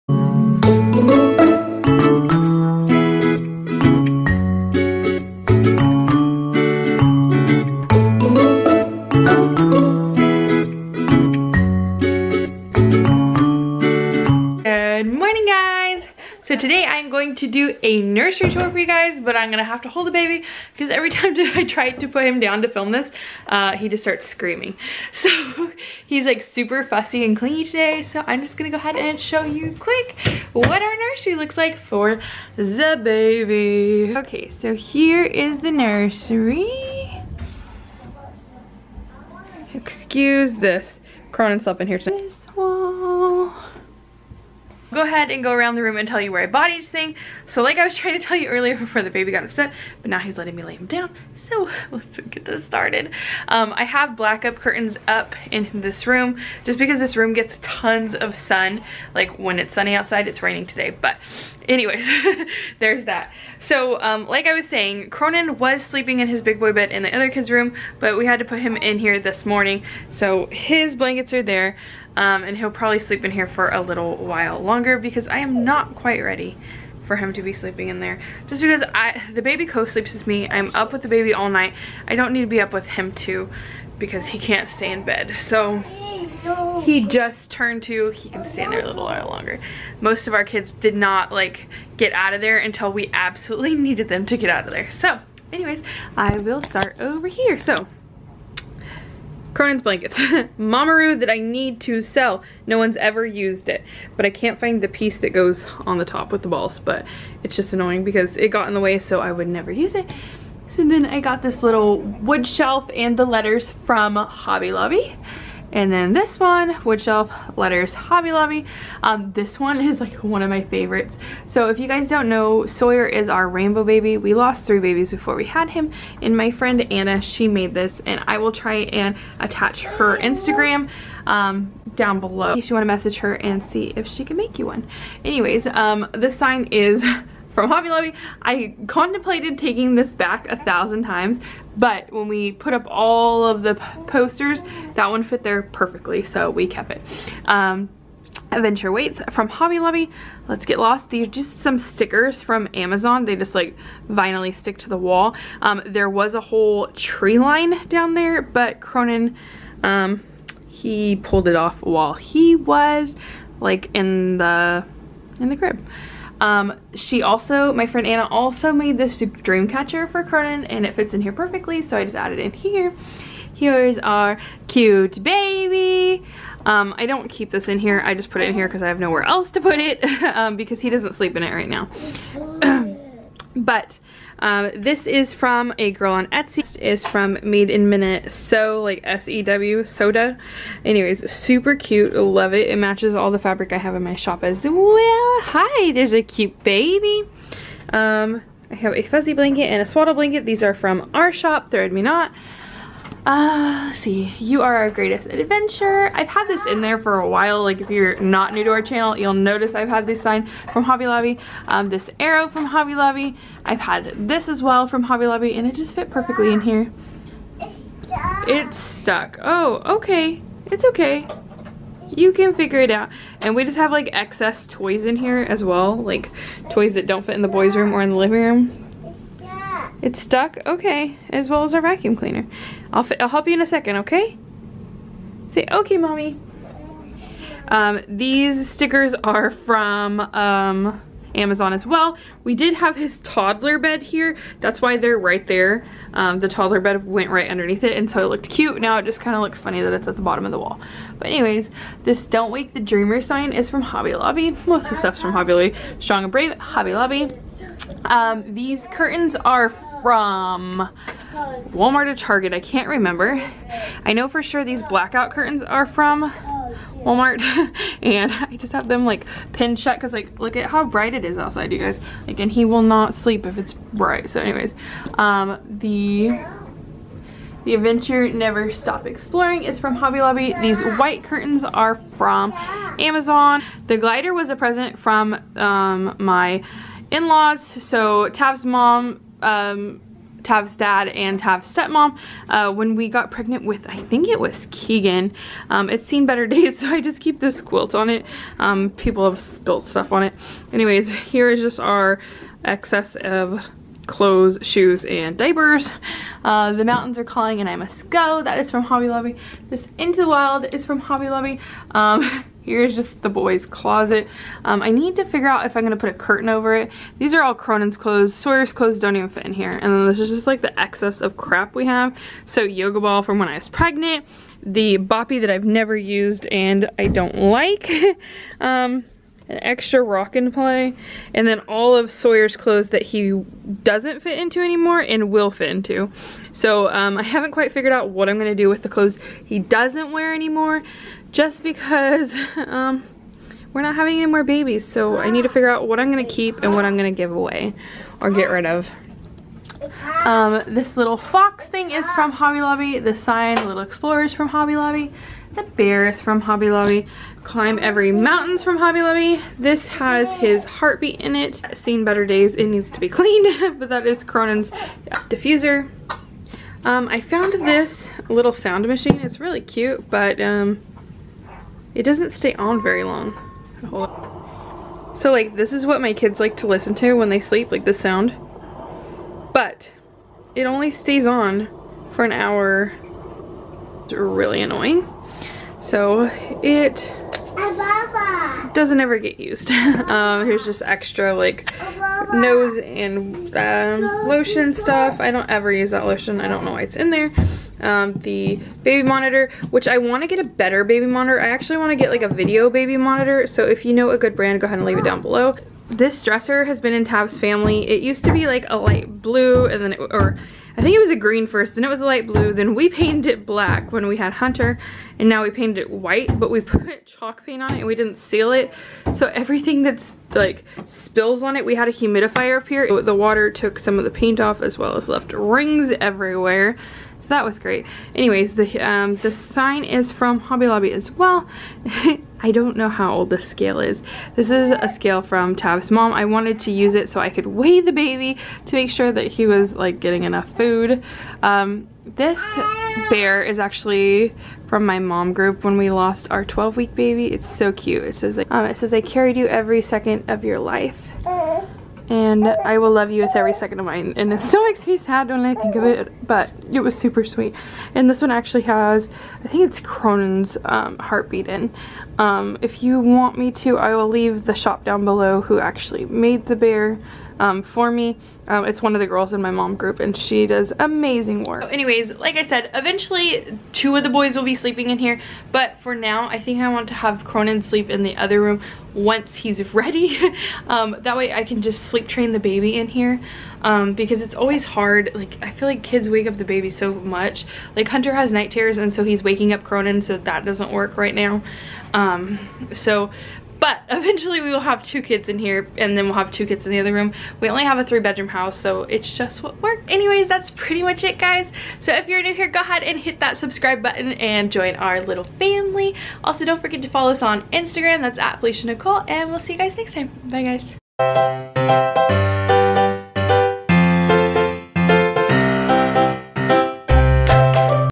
NURSERY REVEAL + TOUR
LONG awaited Nursery Reveal/ Tour for our rainbow baby!
Love this woodland mountain theme

Thank you everyone for following us on this journey!

After suffering 3 consecutive losses, 2 Ectopics and loosing my right tube. I never thought this would be possible. But after every storm comes a rainbow. We are so excited to have officially welcomed our 4th and final boy to our family!

Enjoy a look into our crazy, beautiful life !!

PRODUCTS:

Tommee Tippee Colic:

Swaddle Set for pictures:
Handmade by Banana